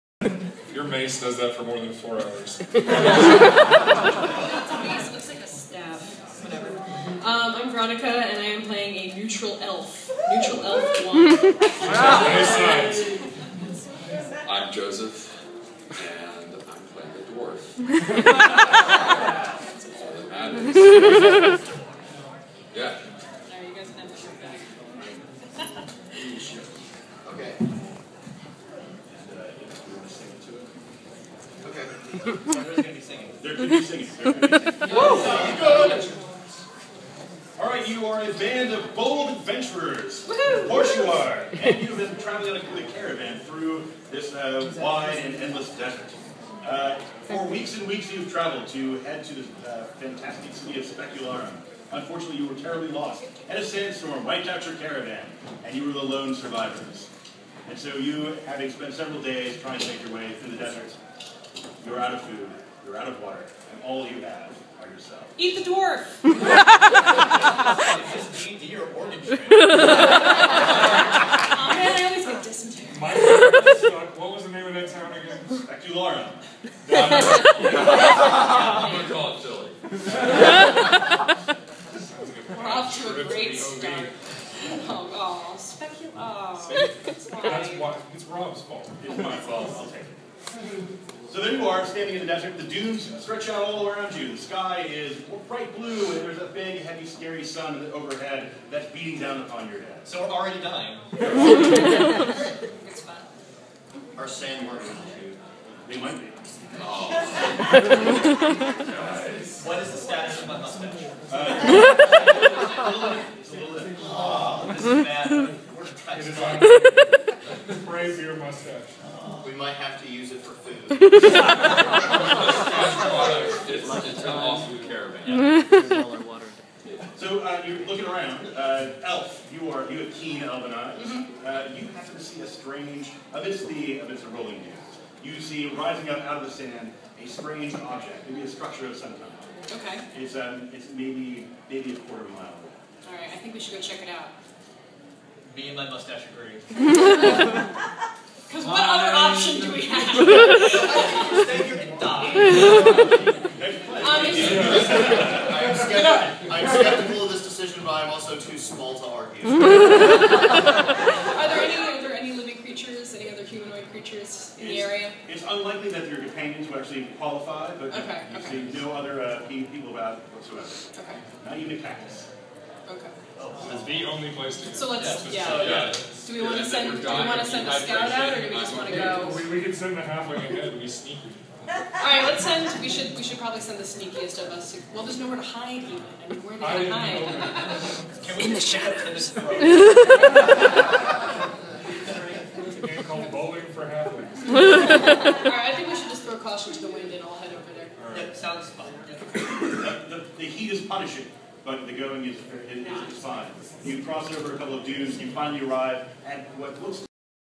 Celebrity D&D
The first three and a half minutes of GMX V.4 Celebrity D&D. I apologize for my terribad laugh.